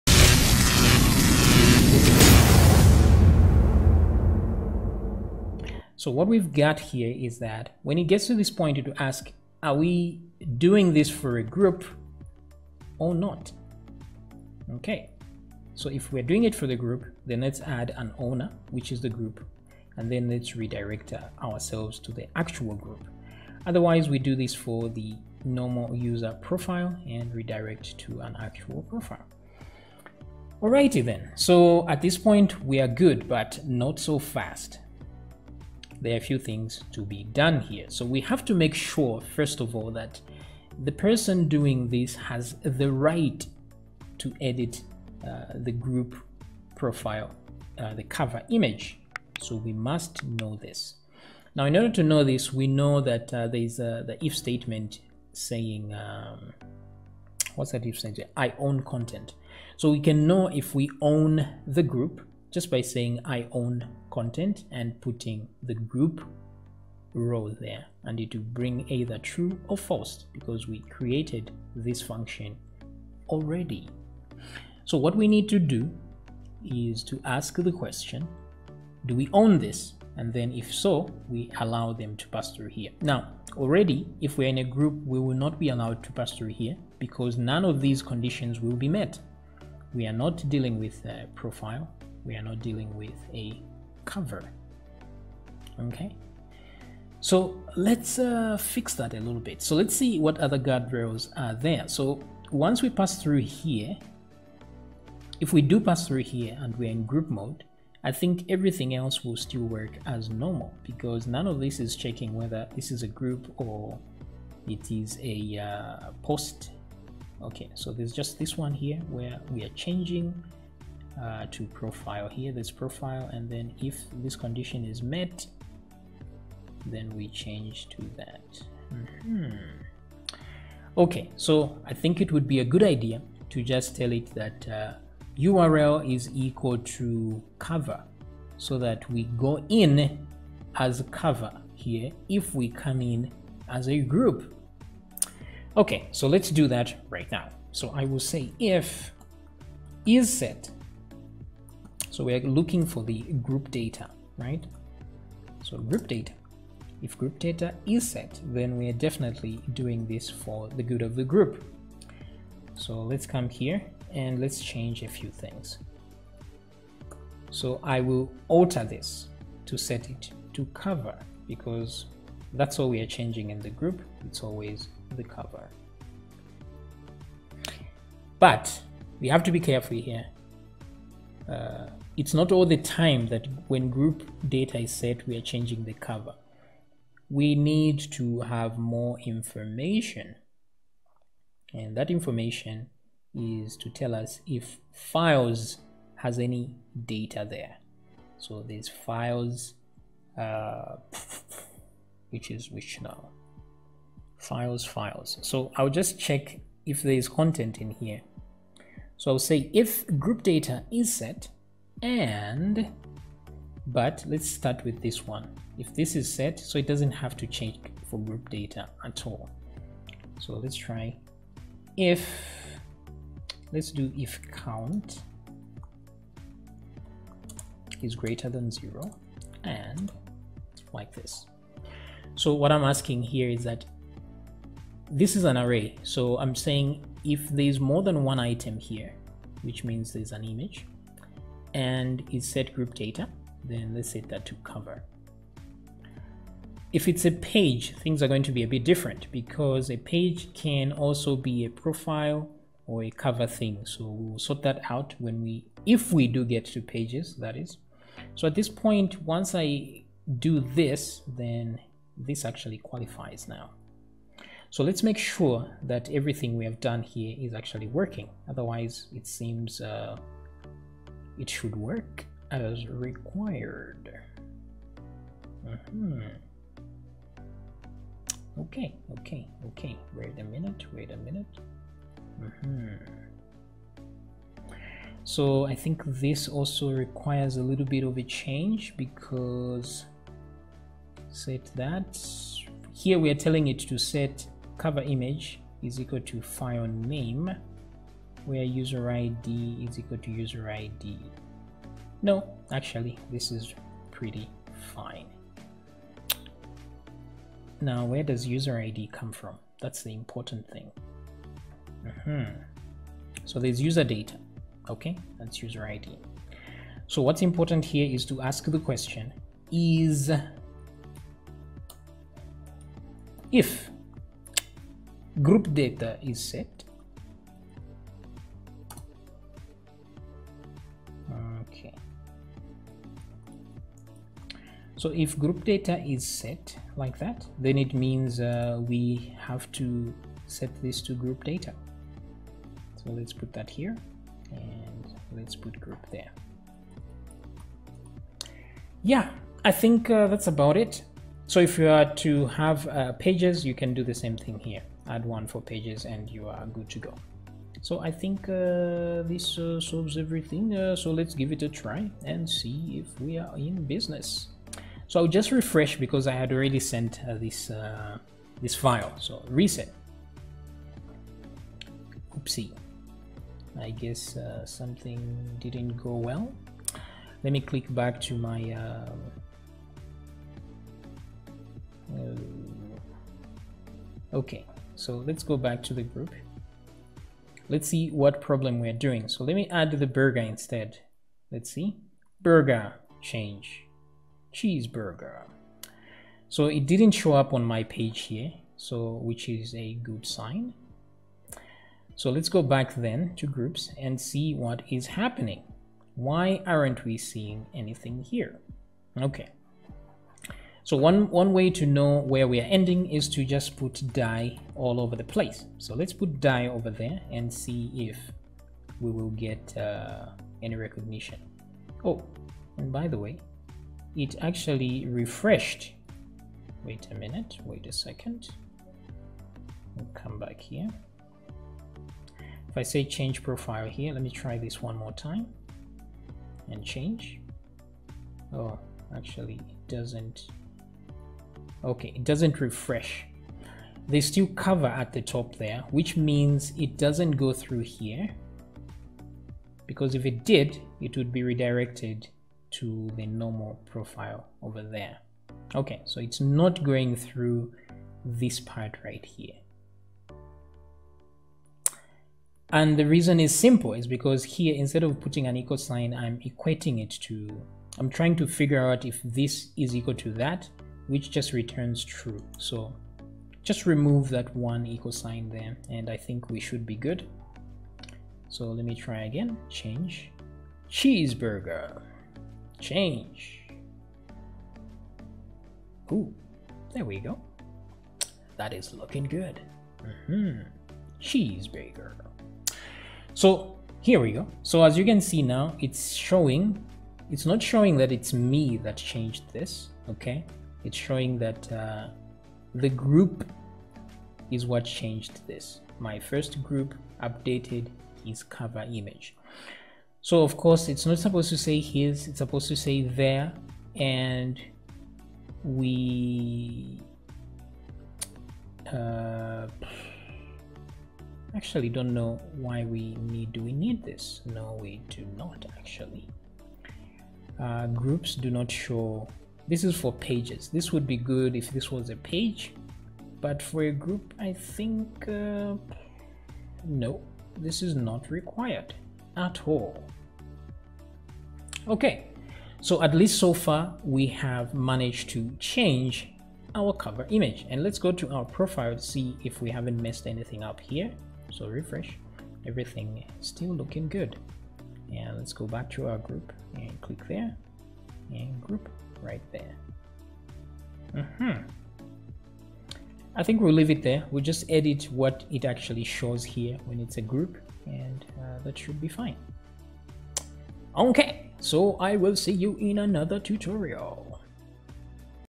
Social Website from scratch - Part 205 - Fixing group cover image 2 | OOP PHP with MYSQL Database
Additional changes and updates to the social website tutorial series, to improve its functionality. we will be adding user groups in this final set of videos